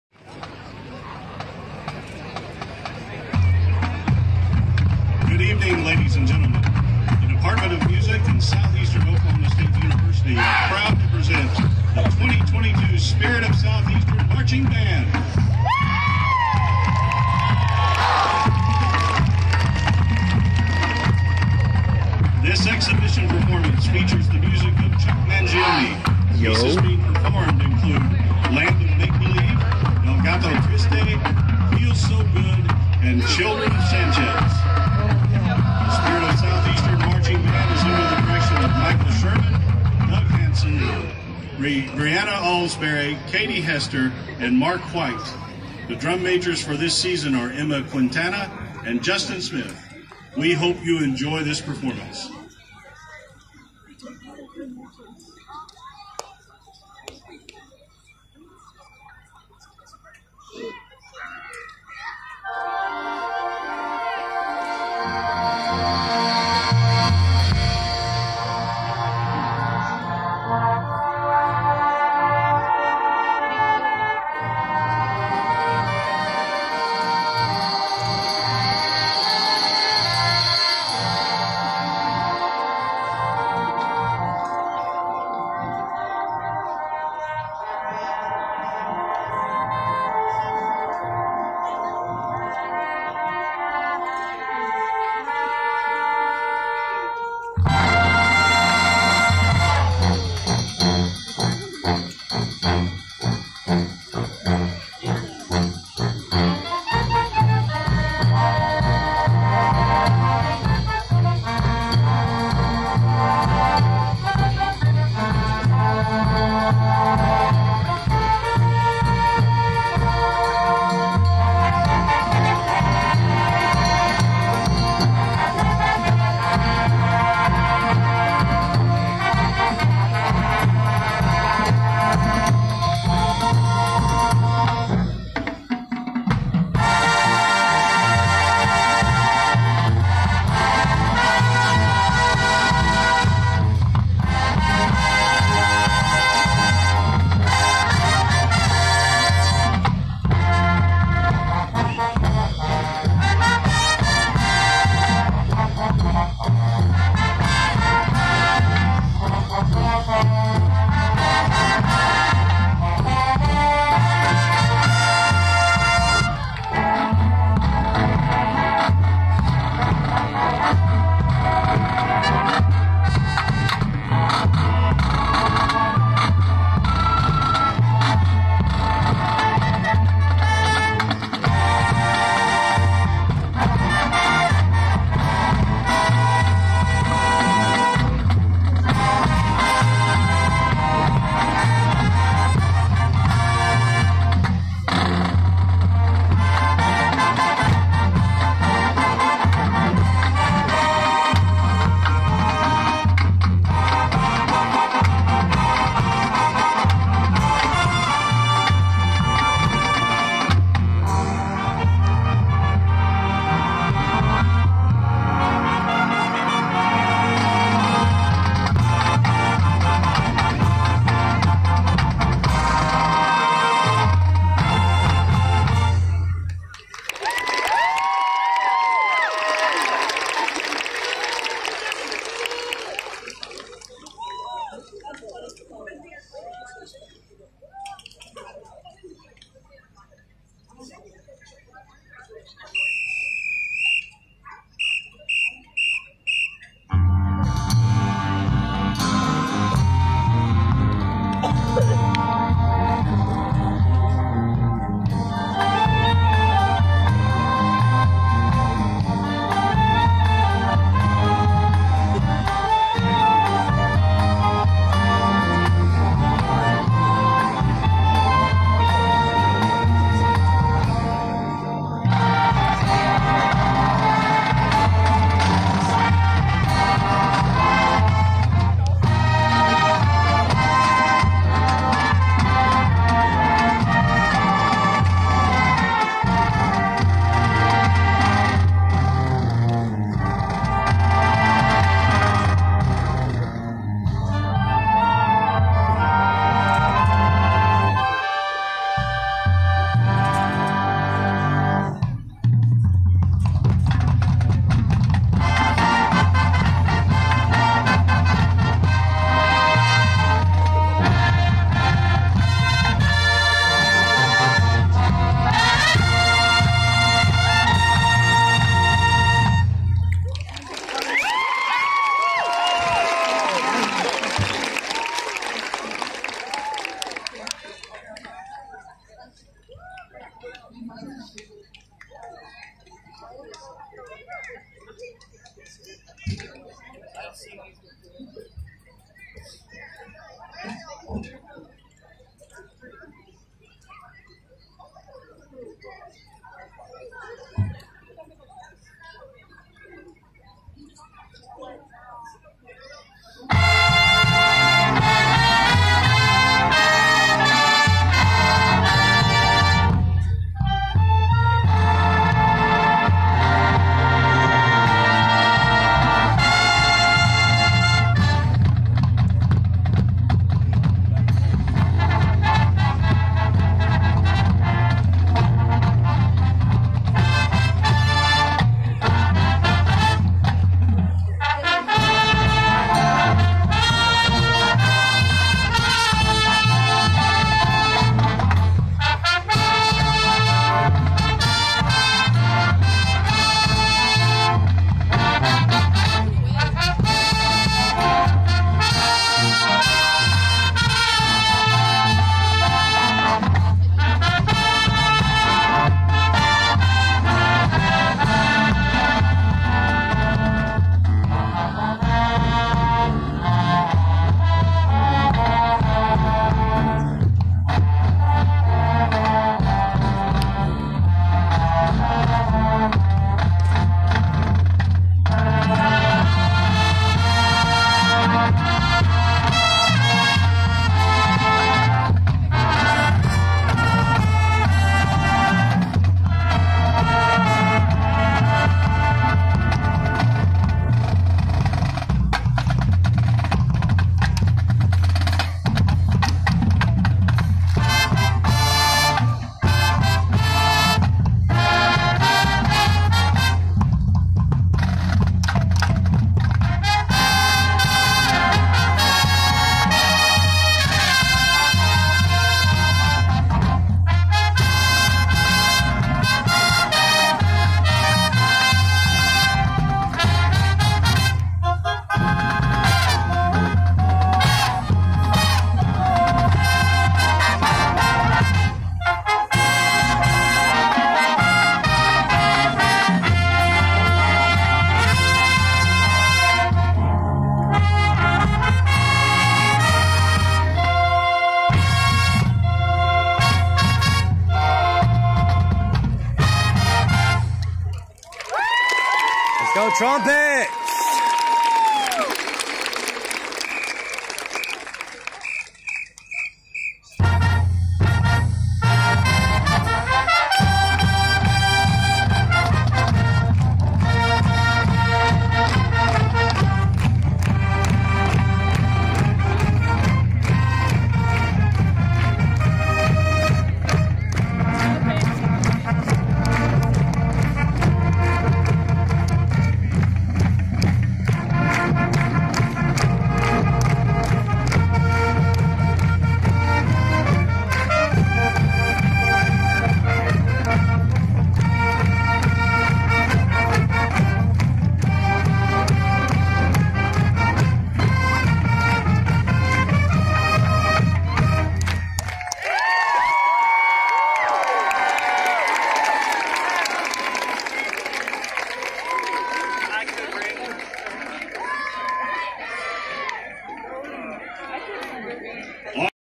South Eastern Oklahoma State University band 2022 - Tribute to Chuck Mangione @ Tishomingo, OK
Sorry for terrible camera work lol